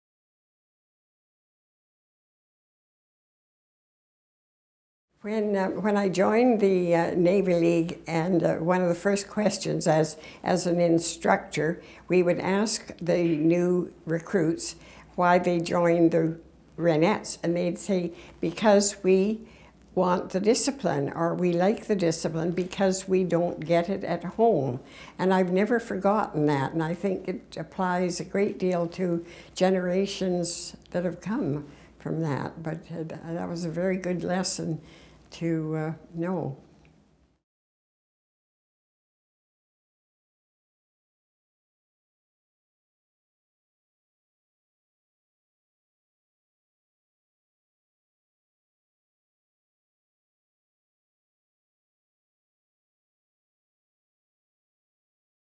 Carroll Strelaeff - Why do you think cadets chose to join the Navy League?
Carroll Strelaeff
Women's Royal Canadian Naval Service, 1953-58
Navy League of Canada, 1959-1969
Images in this video, in order of appearance:
For more information about Research to Remember: In Their Own Words,